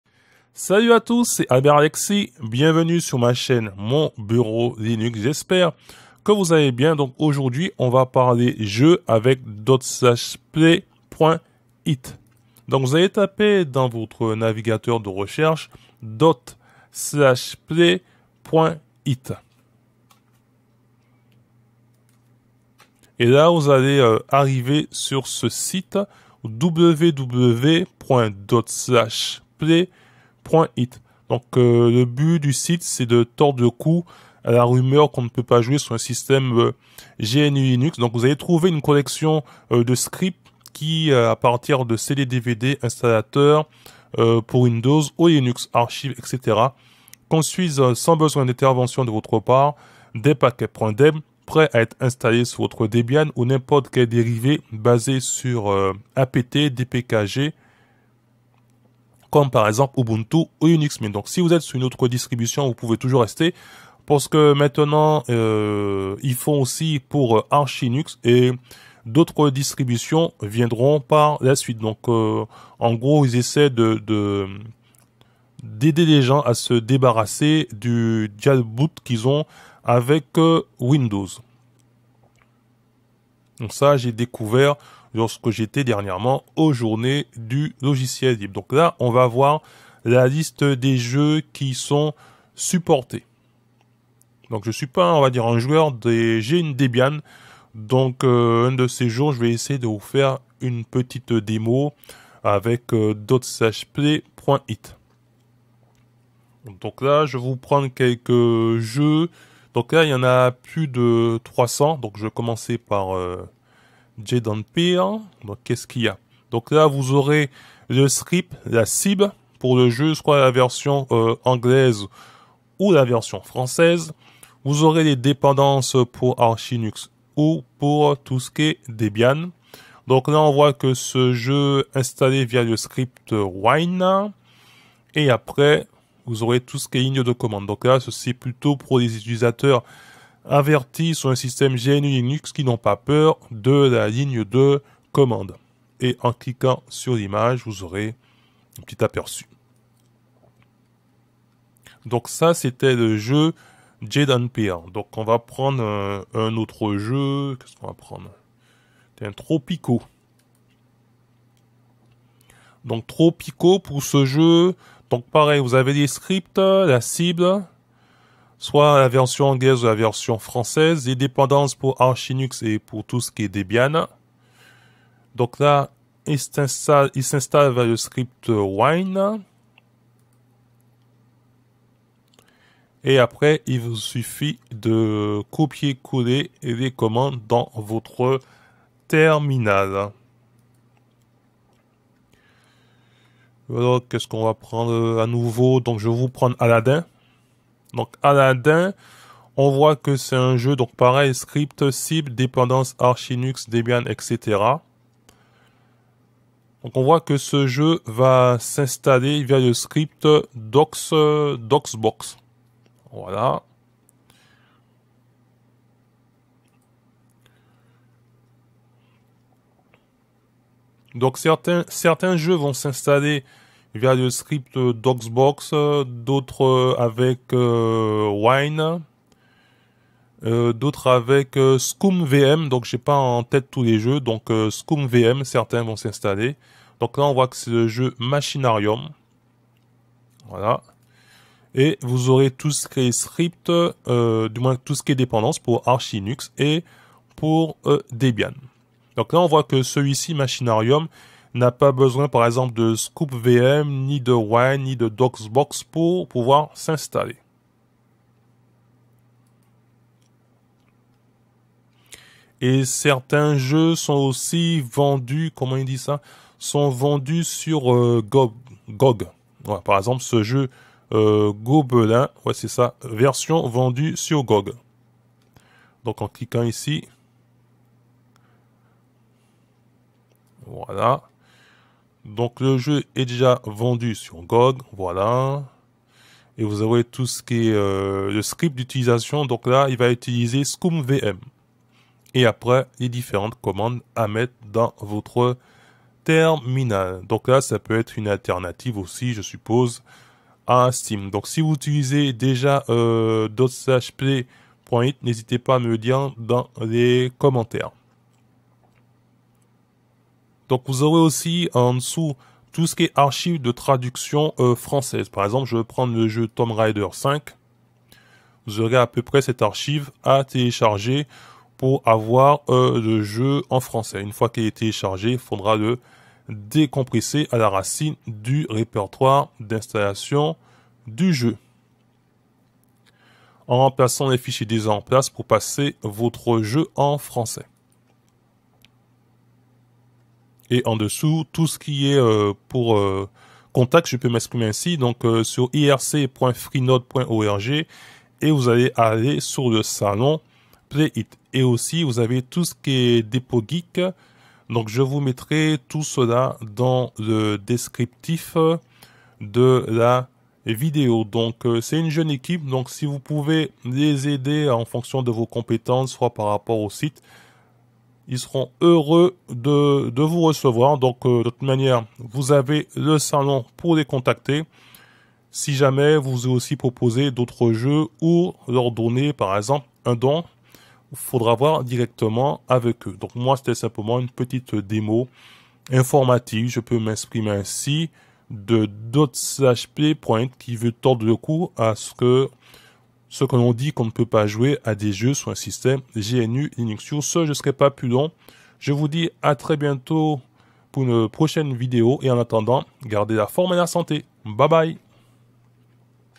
Présentation de ./play.it, à vos jeux ! ( Linux gamin )
Ce site est dédié à un seul but : tordre le cou à la rumeur la plus persistante au sujet de GNU/Linux.
Je parle bien sûr de : « Ton linusque là, c’est nul, il n’y a aucun jeu qui tourne dessus ! ».

salon